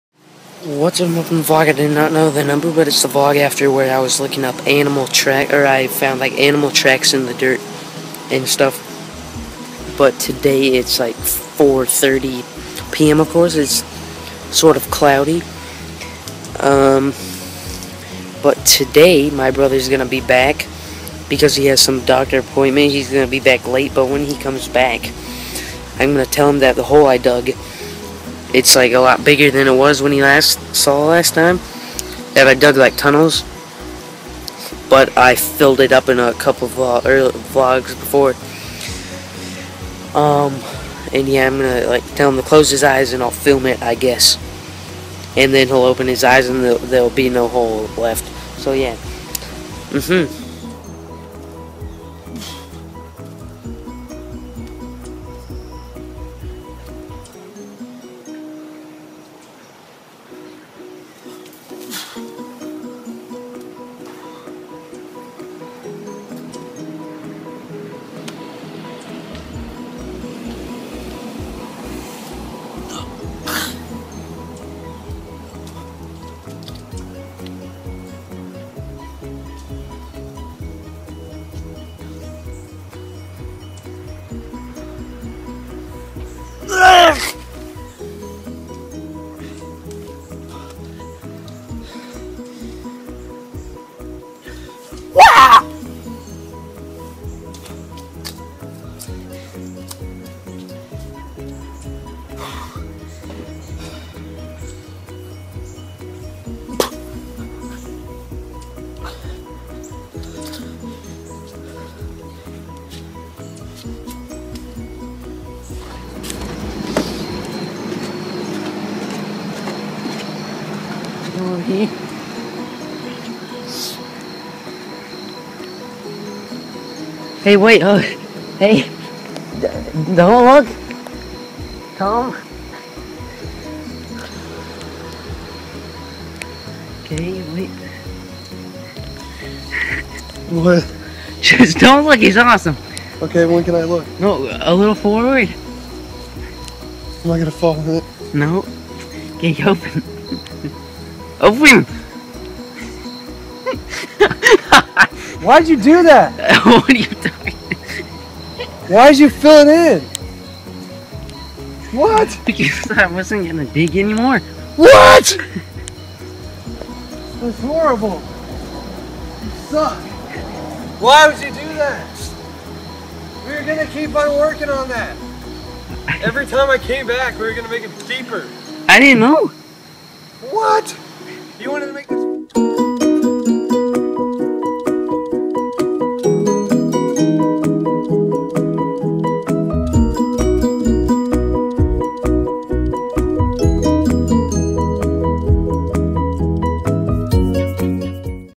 Vlog #176 (Tricking My Brother)
In this video I trick my brother